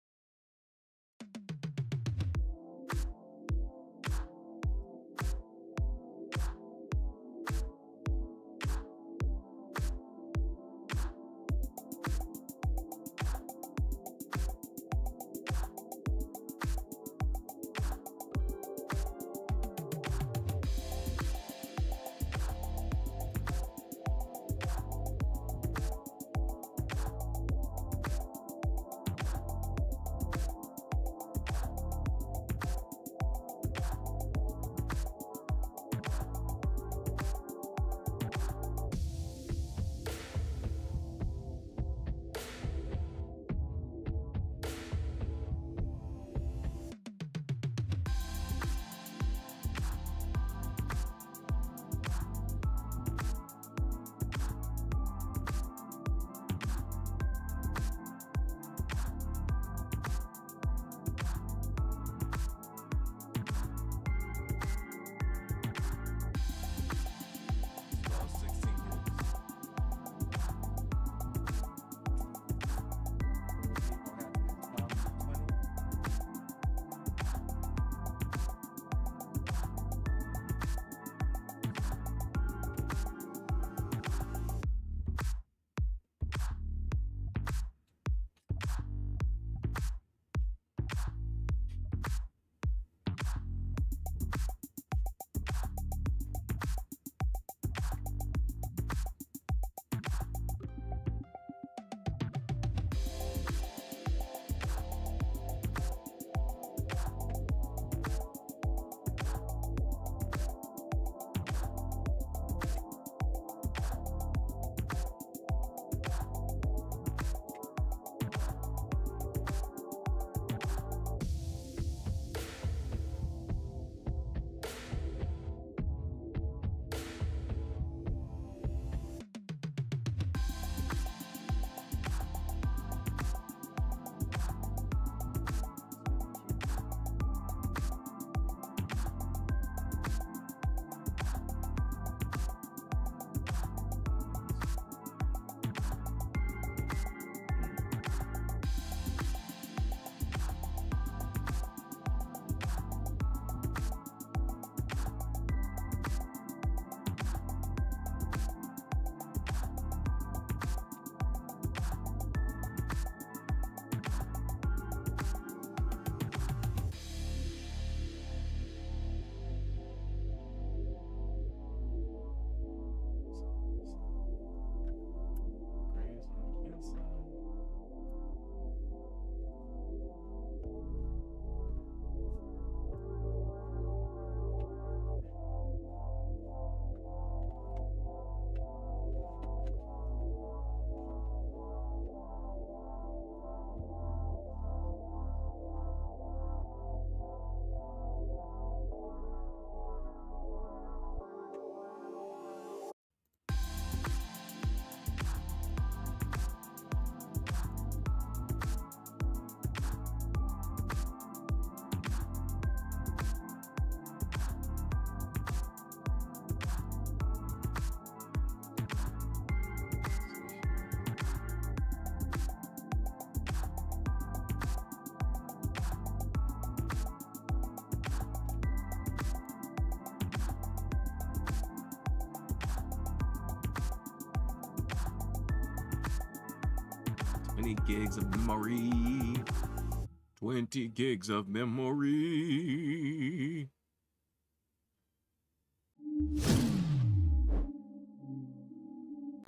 Synology memory upgrade on DiskStation DS920+ NAS, 20GB
Adding memory to Synology 4 bay NAS DiskStation DS920+. Maybe an m.2 is next. More research to do.